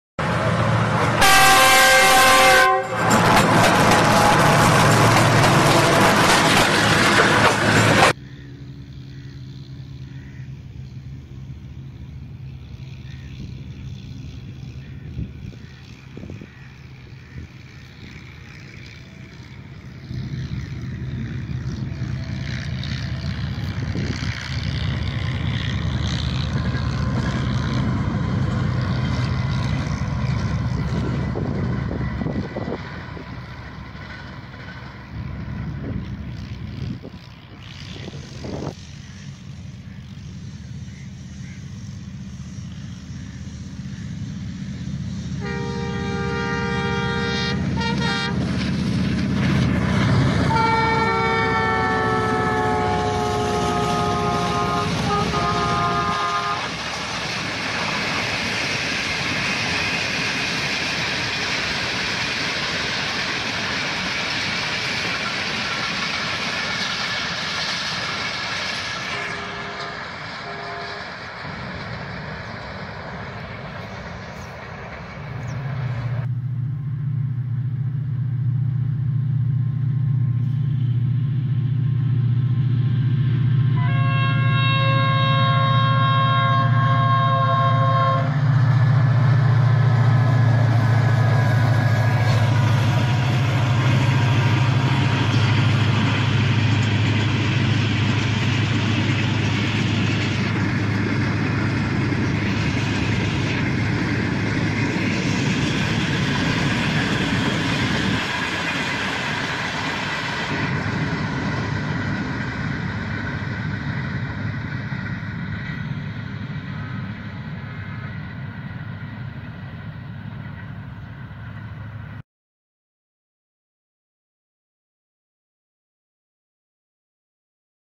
Pakistan Railways || Trains Pass Through Beautiful Mustard field || Lahore
Welcome to Every One
Here Is a Clip Of Pakistan Railways One Light Locomotive Two Express Trains was Passing Through Beautiful Mustard field at green town, Lahore. Green Fields Increase the Beauty Of This Video. In This Video, One Light Locomotive GEU-40-9027 & HGMU-30-8213 With 304 Dn Pak Business Express & ZCU-30-6310 Lead By 8 Dn Tezgam Express.

Details:
Date: 17 & 27 ‎December, ‎2017,
Locomotive: DE GEU-40-9027 || HGMU-30-8213 || ZCU-30-6310
Train: 304 Dn Pak Business Express & 8 Dn Tezgam Express
Place: Green Town, Lahore Punjab